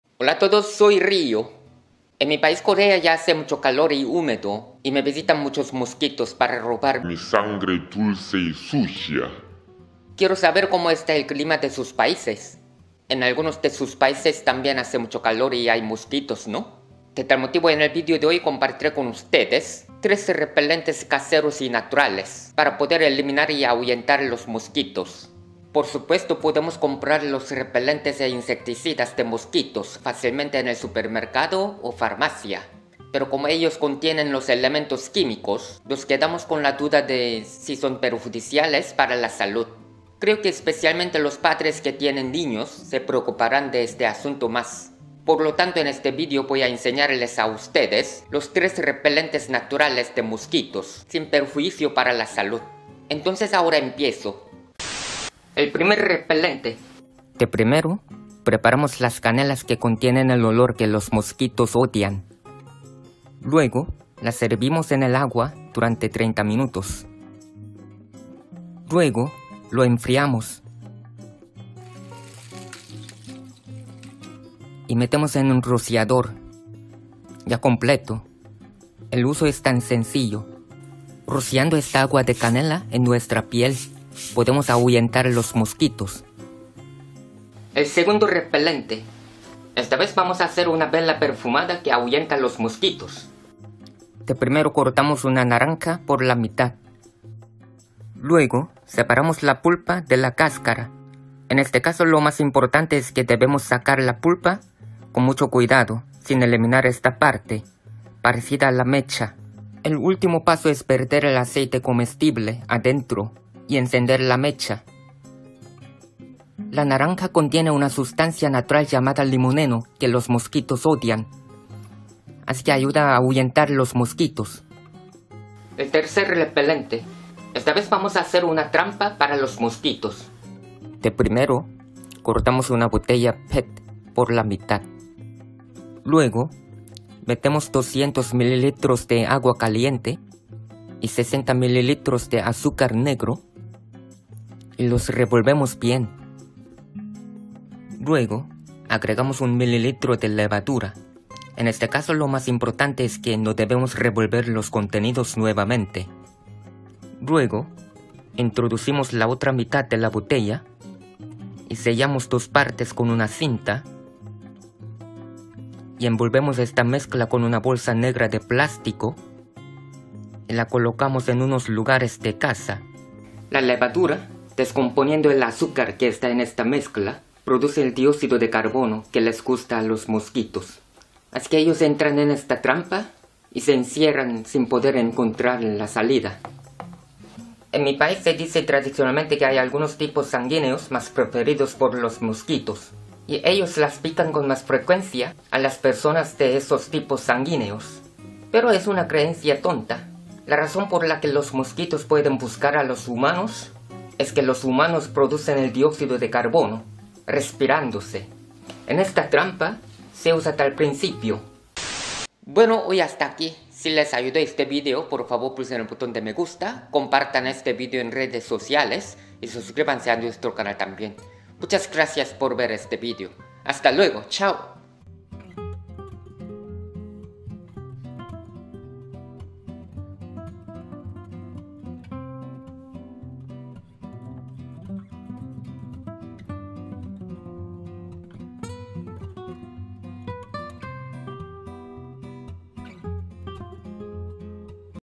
3 Repelentes de MOSQUITOS 100% Naturales y Seguros para tus Niños y Mascotas
Por supuesto podemos comprar los repelentes e insecticidas de mosquitos fácilmente en el supermercado o farmacia.

Pero como ellos contienen los elementos químicos, nos quedamos con la duda de si son perjudiciales para la salud.

Especialmente los padres que tienen ninos se preocuparán de este asunto mas.

Por lo tanto, en el video de hoy vamos a ensenarles los 3 repelentes caseros y naturales para mosquitos sin perjuicio para la salud.